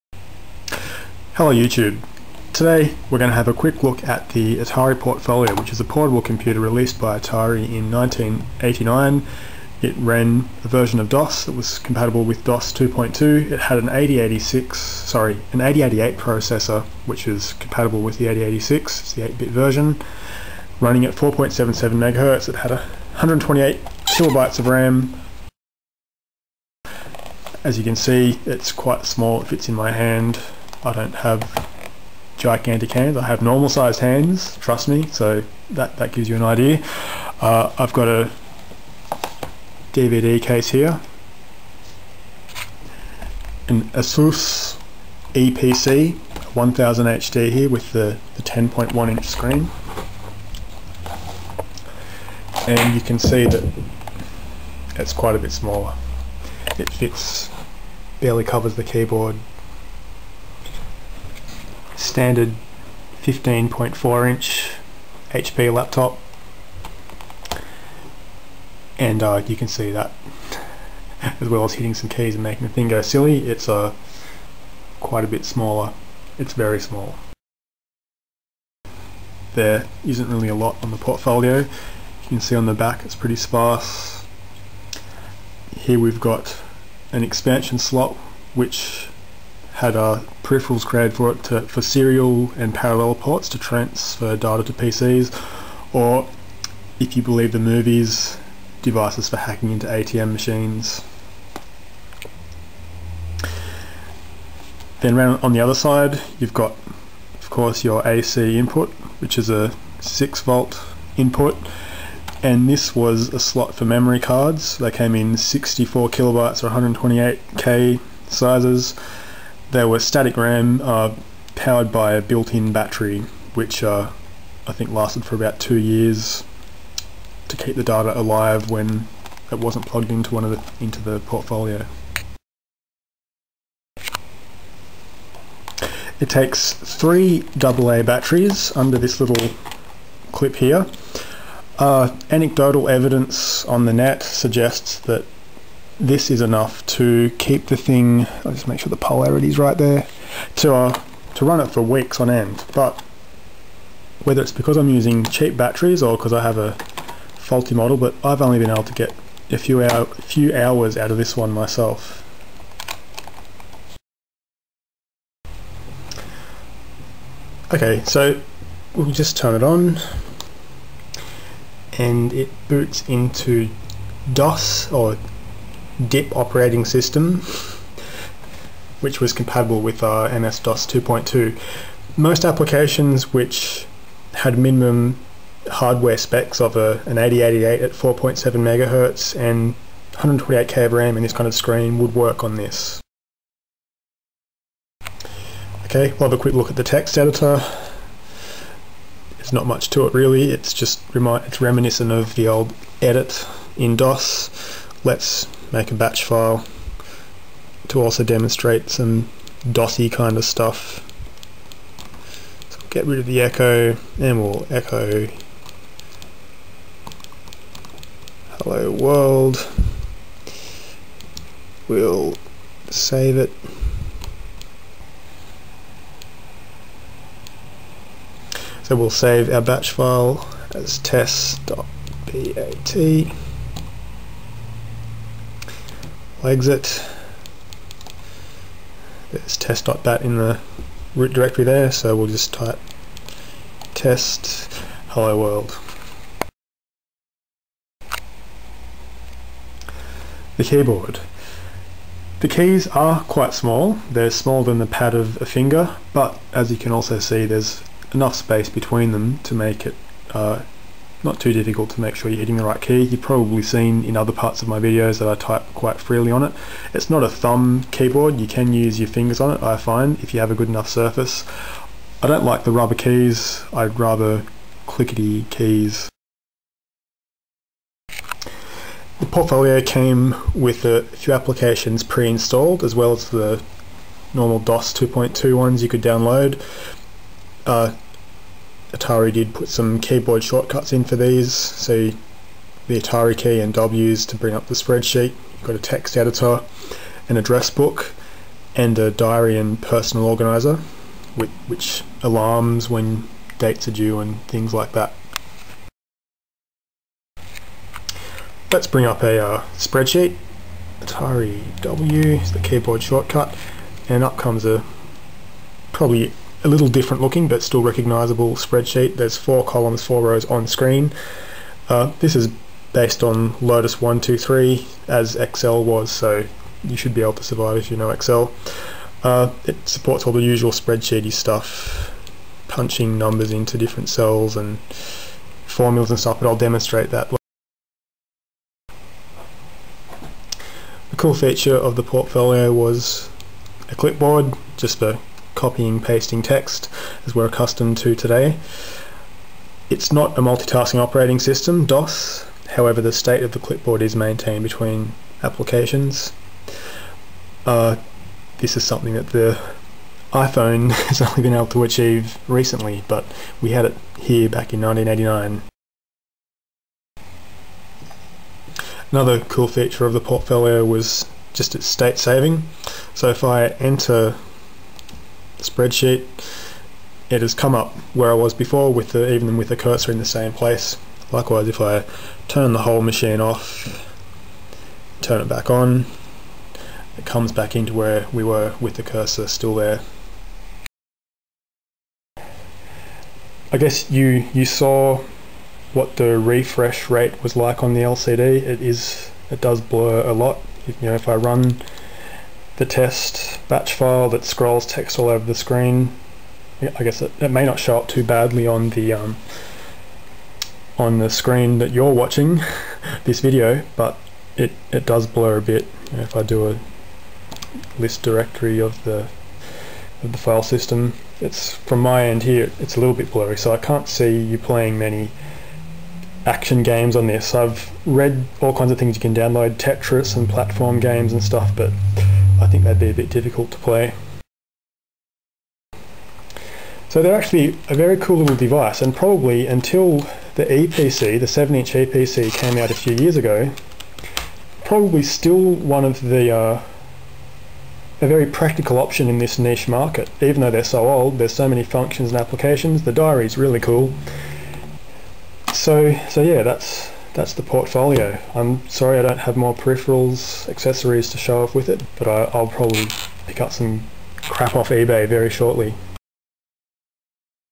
Atari Portfolio - Overview
I made this video to give people a quick tour of the Atari Portfolio, the DOS-based portable computer released by Atari in 1989. It's a bit longer than I'd have liked, but if it helps the contents are as follows:
- An overview in the first 25 secs
- Size comparison with an Eee PC and a normal laptop at 0:25
- Connectivity overview at 1:20
- Installing batteries at 2:15
- Booting up into DIP DOS 2.11 at 2:45
- Using the text editor and creating a batch file at 3:15
- Assessment of the keyboard at 4:10
- An overview of the preinstalled applications at 4:40
- A peek at the spreadsheet app at 5:10
- A look at functions of the clipboard, state saving, and screen from 5:40 to 7:15
- Conclusions at 7:20

I'll probably do two more videos, one using some more advanced formula's and playing with the spreadsheet, and in another we might get some more interesting batch files happening. We'll see!

Thanks to Roger from MACE (Melbourne Atari Computer Enthusaists) for use of the Portfolio.